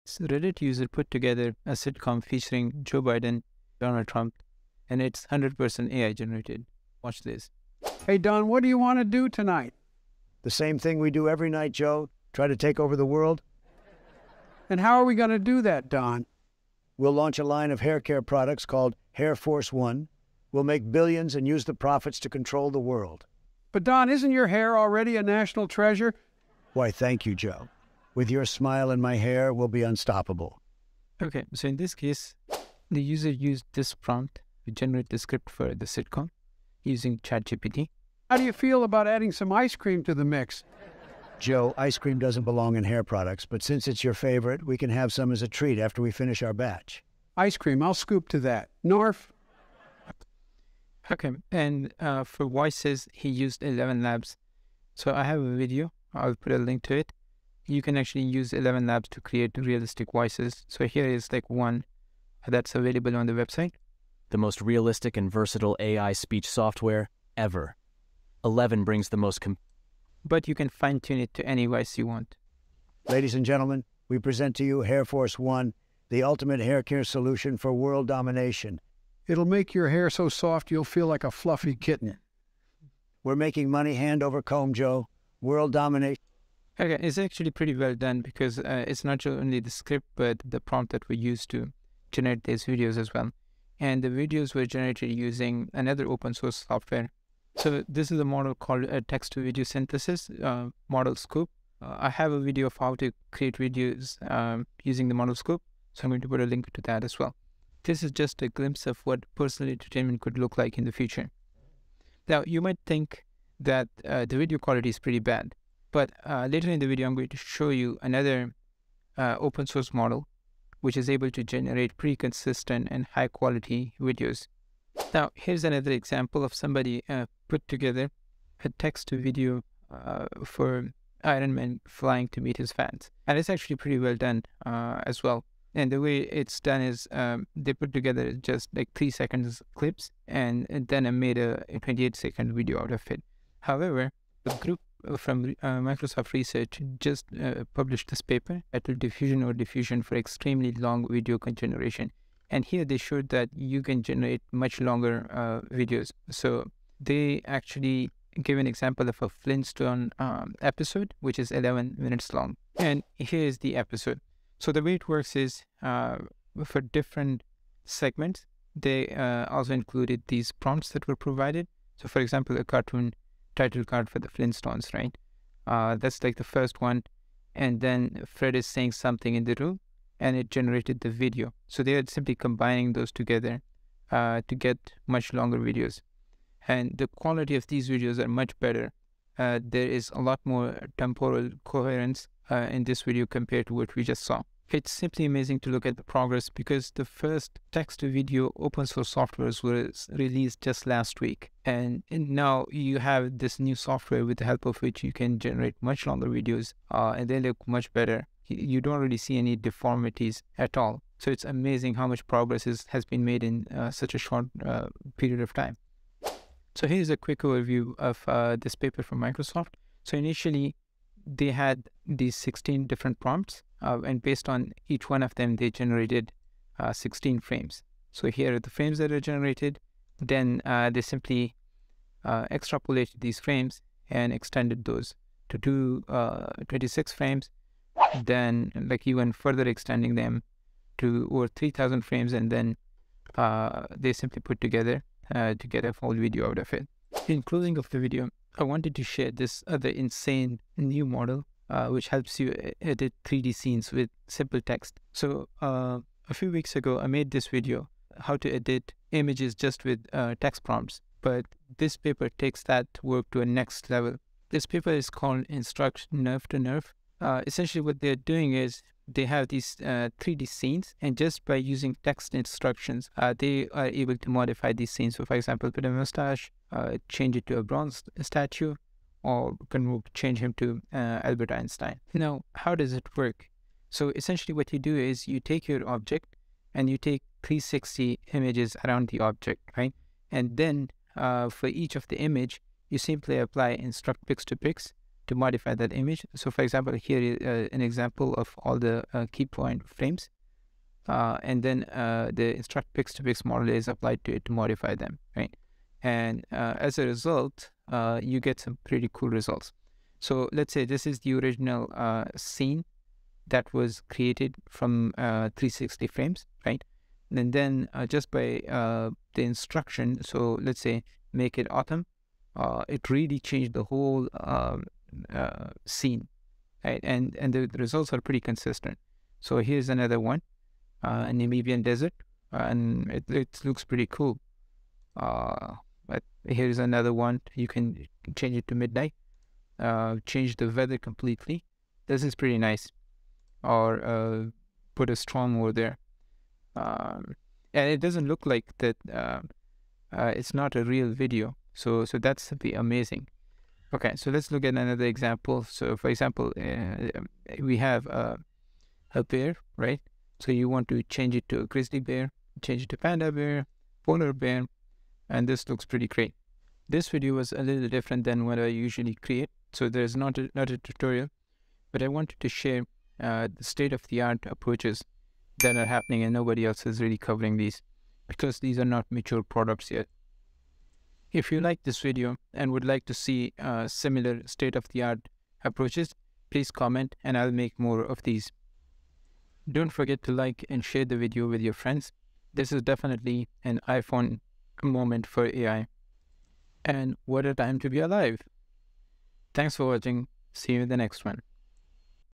AI Made This SITCOM! ft. ElevenLabs, AI Text to Videos, Instruct-NeRF2NeRF
Putting together the current advancements text to speech (with elevenlabs), text to video (using modelscope), and 3D scene editing with instructions. I will show you the exciting applications of these technologies by community members. One of the Reddit users  put these together to create a 100% AI-generated SitCom of Joe Biden and Donald Trump. Enjoy :)